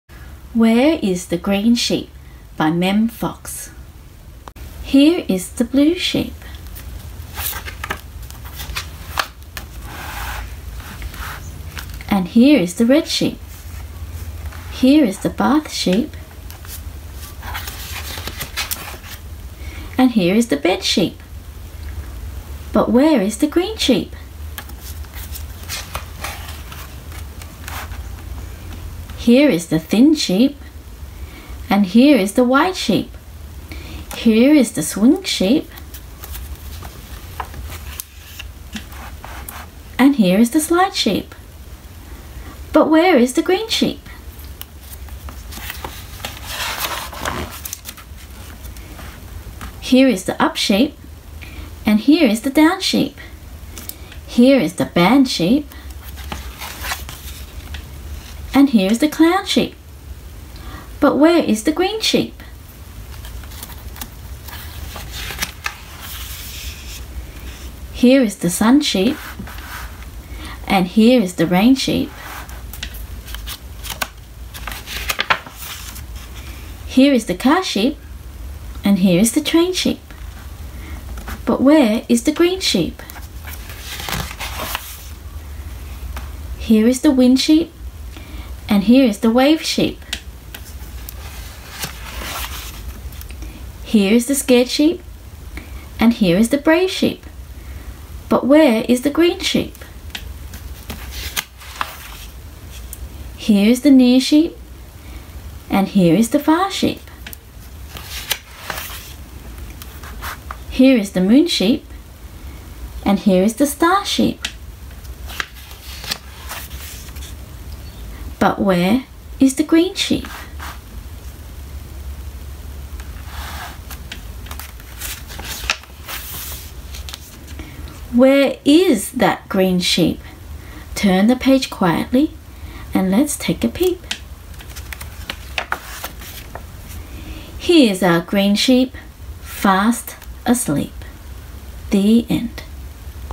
Kids Book Story time: Read Along Where is the green sheep?
New stories every couple days.
Children's read along story
Where is the green sheep?
Fun kids stories to read along to.
Story time made easy.